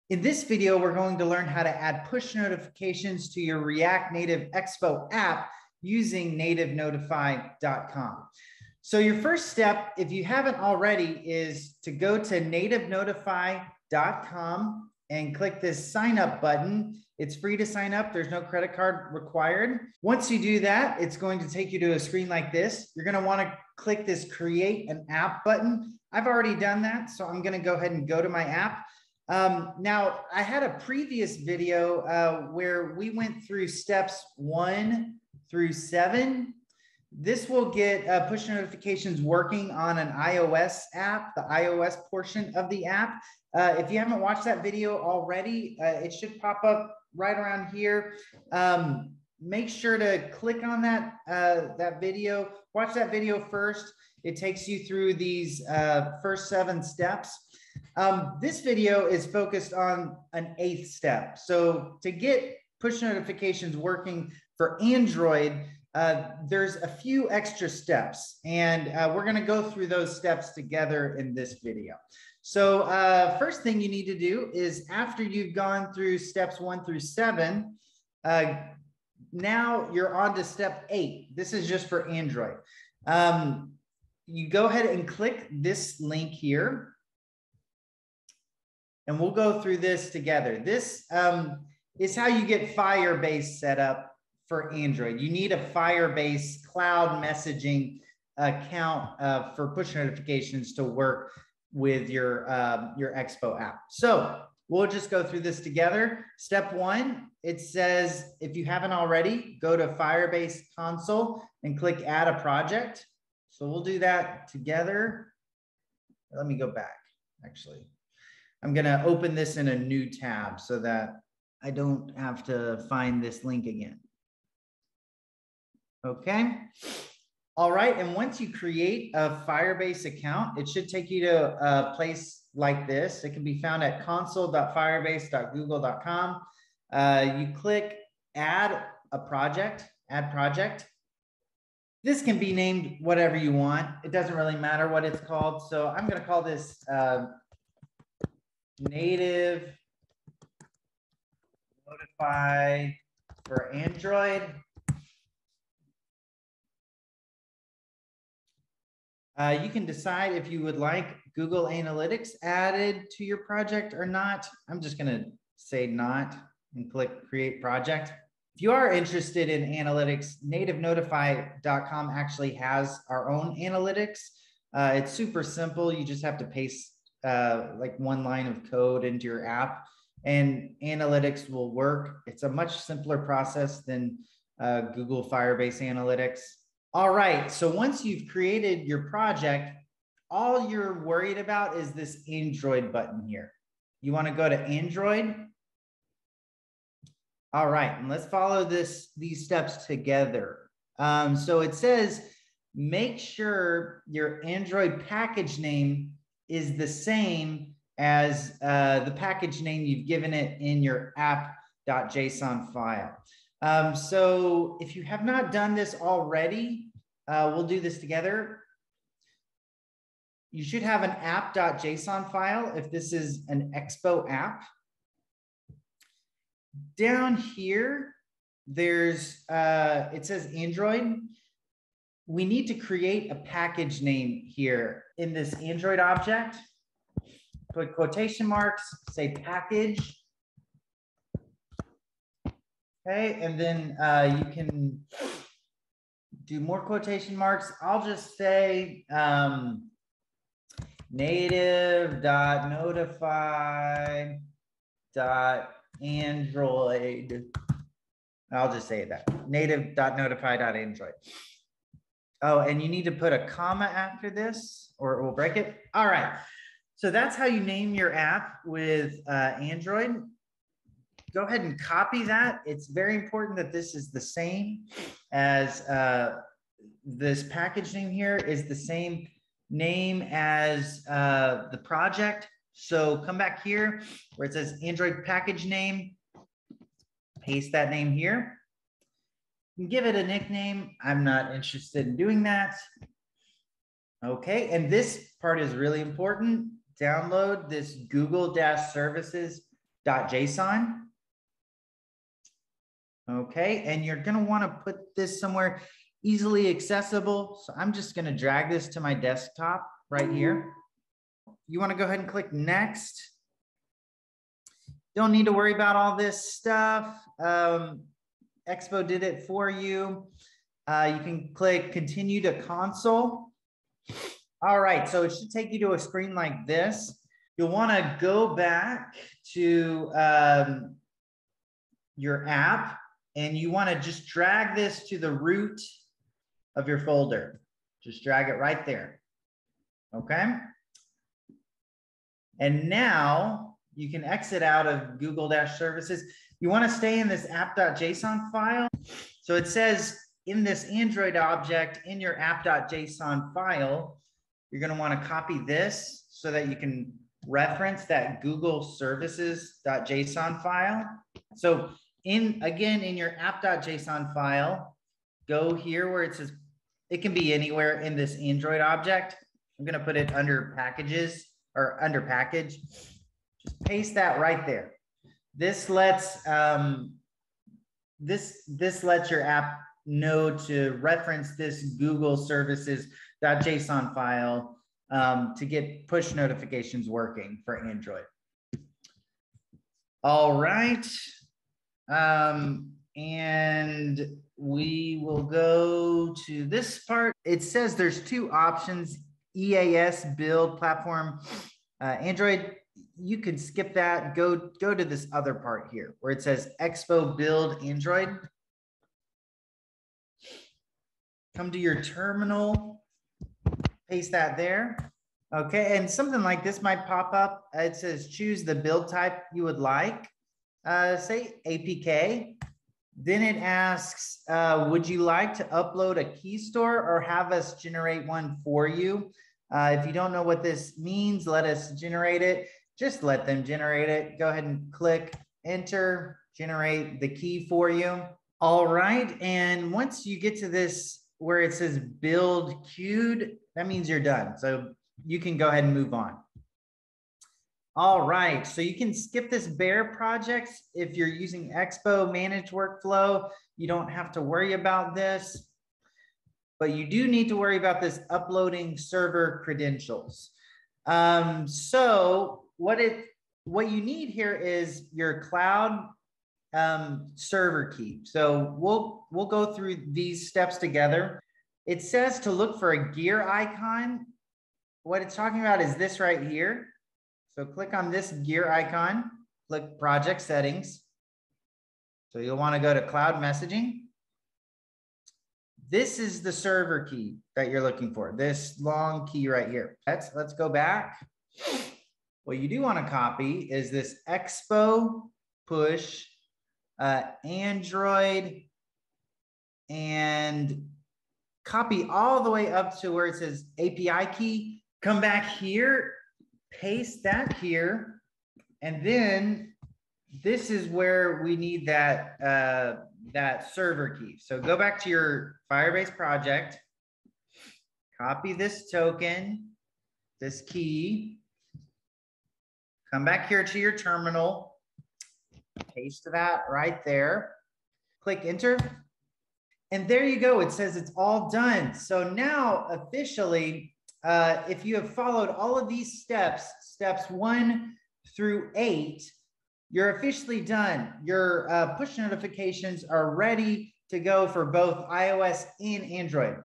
Android Push Notifications | Expo | React Native
How to add Android Push Notifications to your React Native Expo app using NativeNotify.com. Learn how to create and add Google Firebase credentials to your React Native Expo app. Google Firebase credentials are required to get push notifications working in React Native Expo apps.